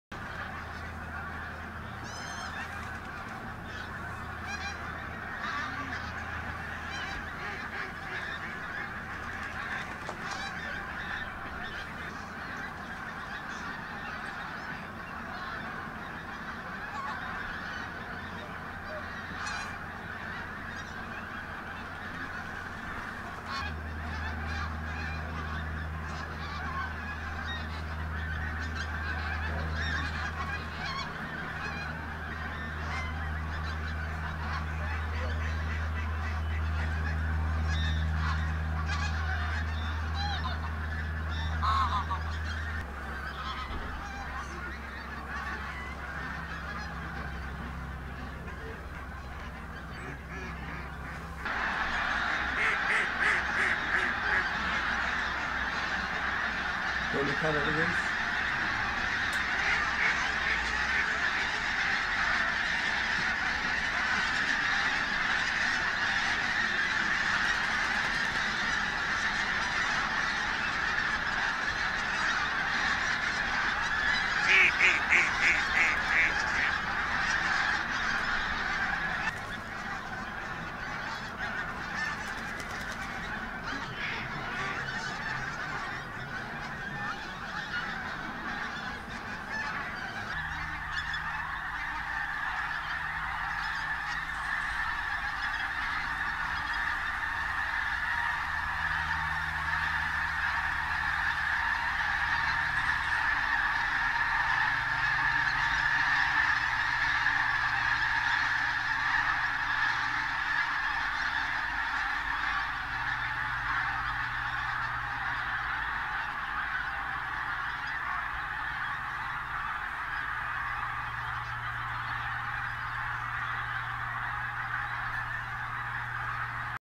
Pink- Footed Geese - Martin Mere WWT Reserve, Lancashire, October 8th. 2012
A nice bright day for a change, and a lot of mobile geese in and out of the Reserve.